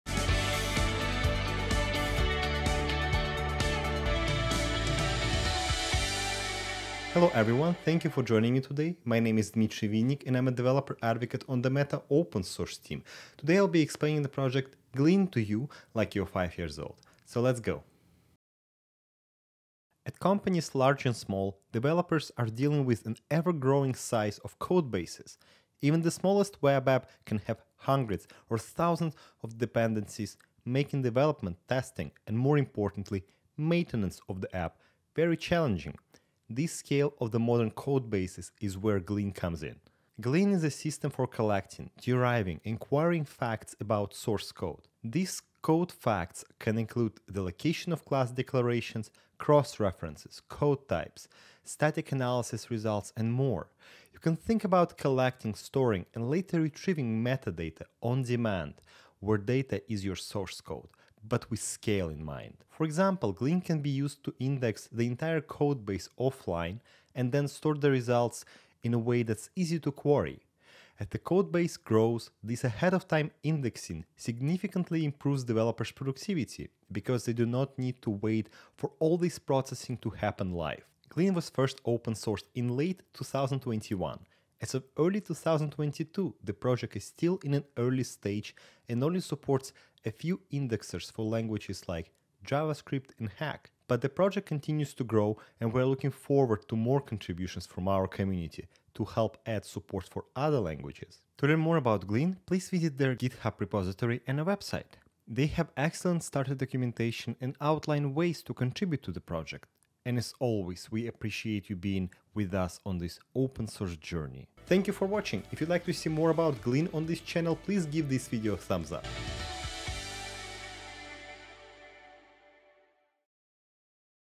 ELI5: Glean
In this video, Meta Open Source Developer Advocate Dmitry explains Glean, an open source system for collecting, deriving and querying facts about source code.

Project Links

Social Accounts